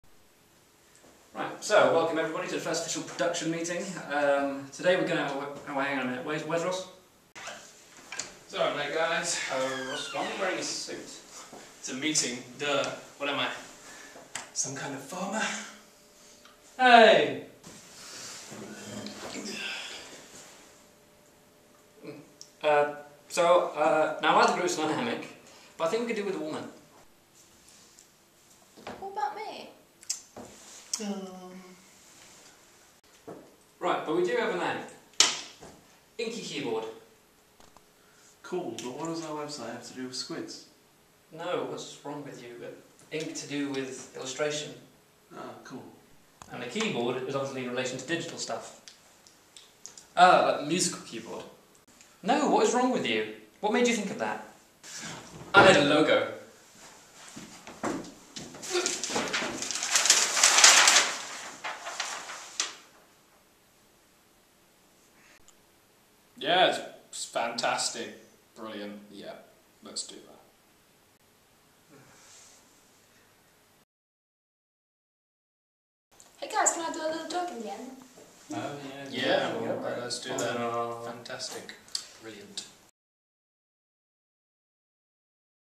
Production Meeting
Our first meeting filmed for you all to see

featuring the entire cast:
Matt, Ross, Brad, Ana and Dan.

Thanks to edd for being our camera man :D

Written, directed and edited by Ross